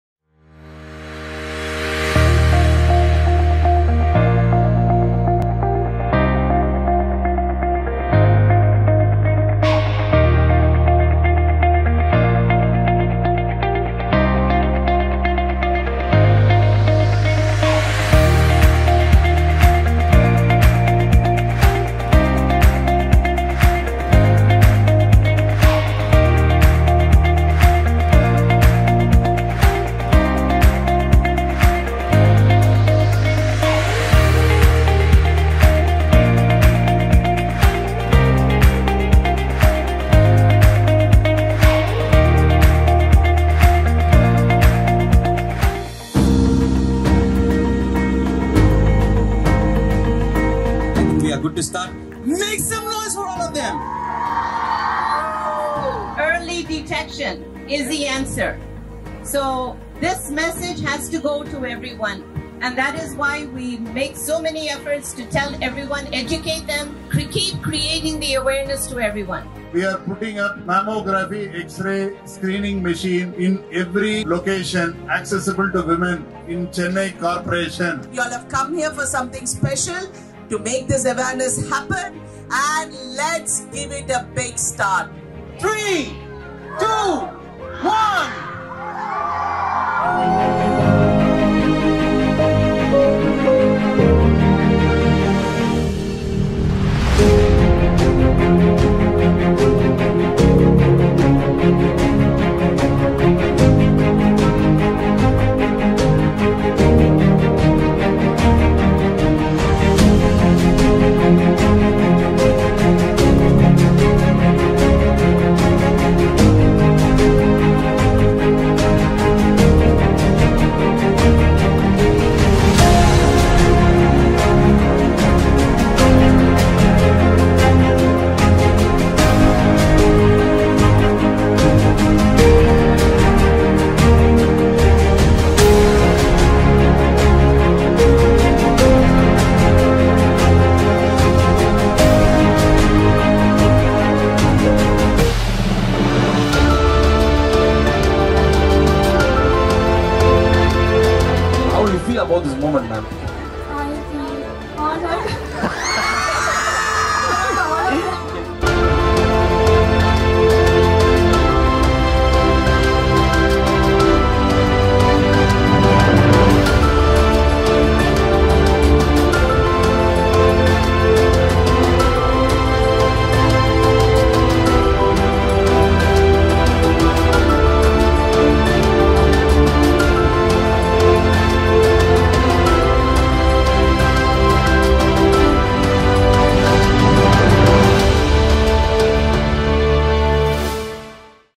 One Walk One Hope - Breast Cancer Awareness Walk 2022
✨Greetings from CANSTOP!

🎗COME OCTOBER. THINK PINK.

To commemorate International Breast Cancer Awareness Month, "CAN-STOP" organised several activities throughout the month of October to raise awareness of the importance of regular breast examinations among the general public. CANSTOP organized its 13th annual walkathon event called "ONE WALK ONE HOPE" to raise public awareness about breast cancer on October 30th, 2022 at 🏫 Olcott Memorial School, Besant Nagar. The students' schools and colleges took part, while companies and NGOs, along with the general public, participated in the walkathon. More than 3500 people took part in this walkathon.

Mrs. Radikaa Sarathkumar, Actress & CANSTOP- Ambassador, and Rtn. Nandakumar, District Governor of RID 3232, were the chief guests.

The walk stresses the importance of early detection and self-breast examination with the strong message "Hope Starts with ME".
Hope starts with us. ✨

Thanks to all the volunteers for supporting a great cause and for contributing your valuable time with us.

Regards
CANSTOP - Team